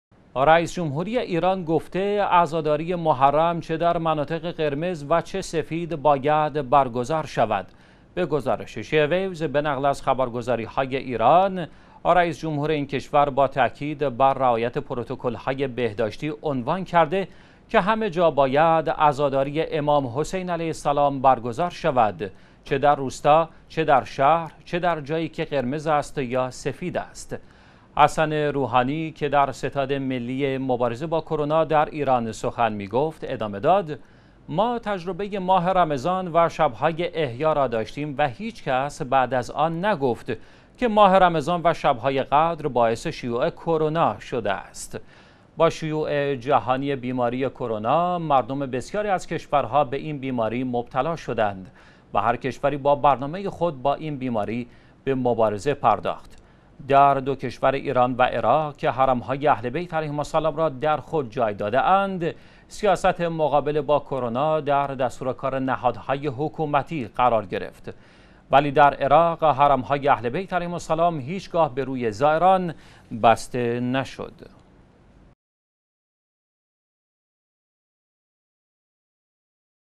رئیس جمهور ایران: مجالس عزای حسینی باید برگزار شوند
رئیس جمهوری ایران گفته عزاداری محرم چه در مناطق قرمز و چه سفید باید برگزار شود.
به گزارش شیعه ویوز به نقل از خبرگزاری های ایران، رئیس جمهور این کشور با تاکید بر رعایت پروتکل های بهداشتی عنوان کرده که همه جا باید عزاداری امام حسین علیه السلام برگزار شود، چه در شهر و چه در روستا، چه در جایی که قرمز است یا سفید است.
حسن روحانی که در ستاد ملی مبارزه با کرونا در ایران سخن می گفت، ادامه داد: ما تجربه ماه رمضان و شب‌های احیا را داشتیم و هیچکس بعد از آن نگفت که ماه رمضان و شب‌های قدر باعث شیوع کرونا شده است.
با شیوع جهانی بیماری کرونا، مردم بسیاری از کشورها به این بیماری مبتلا شدند و هر کشوری با برنامه خود با این بیماری به مبارزه پرداخت.
در دو کشور ایران و عراق که حرم‌های اهل بیت علیهم السلام را در خود جای داده اند، سیاست مقابله با کرونا در دستور کار نهادهای حکومتی قرار گرفت.
ولی در عراق حرم های اهل بیت علیهم السلام هیچگاه به روی زائران بسته نشد.